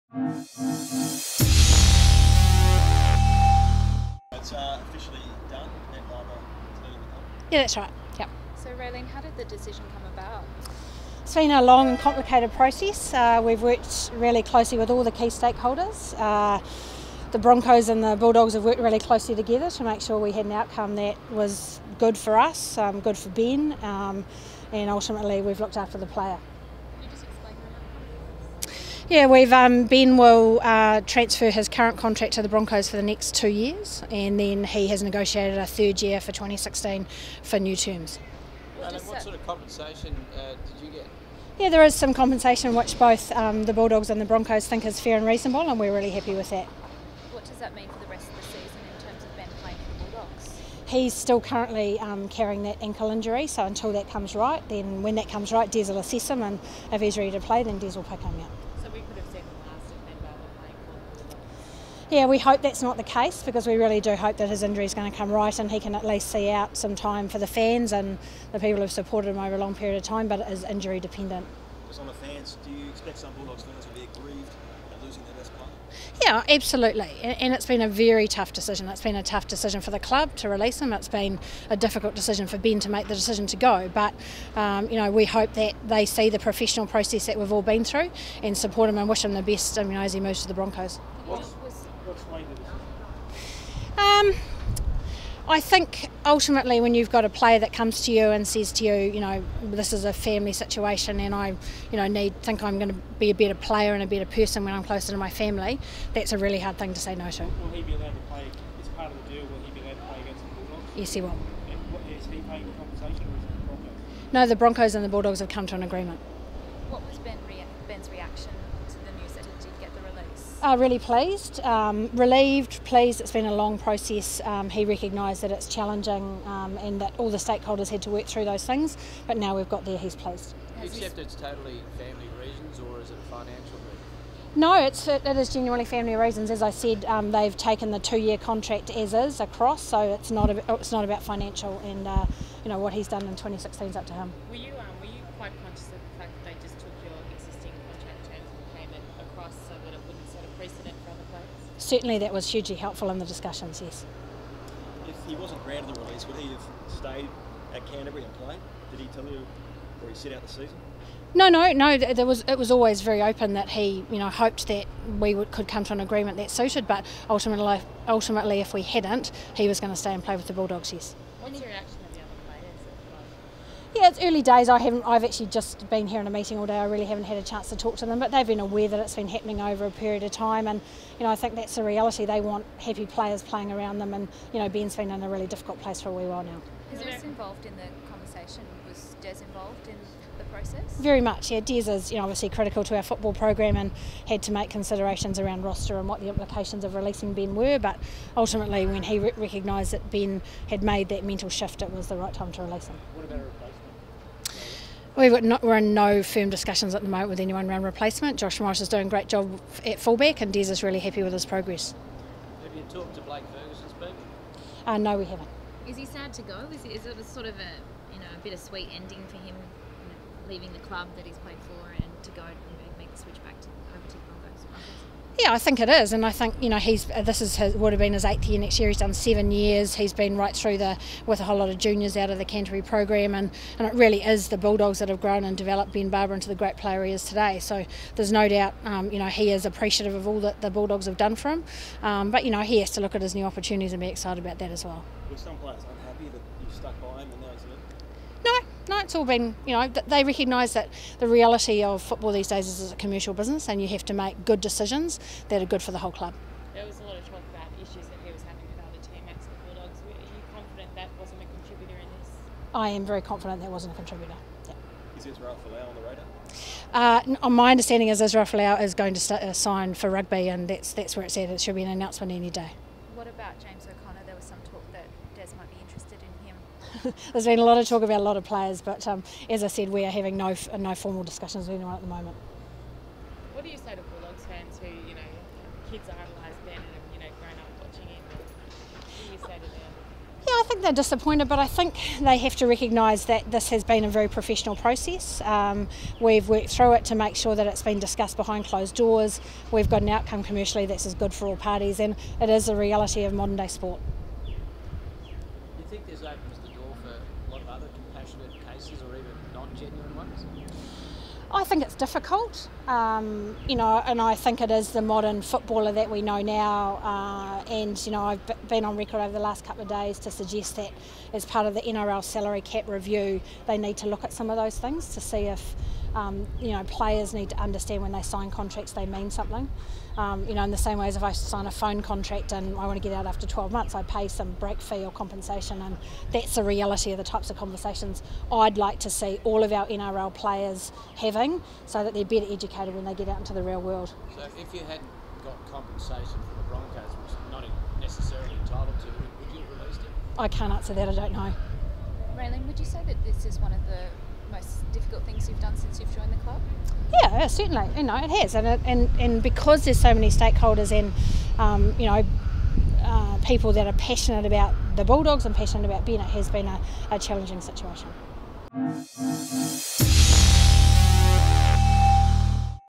Bulldogs release Ben Barba
The Canterbury Bankstown Bulldogs have today agreed to Ben Barba's request that he be released from the final two years of his contract on compassionate grounds.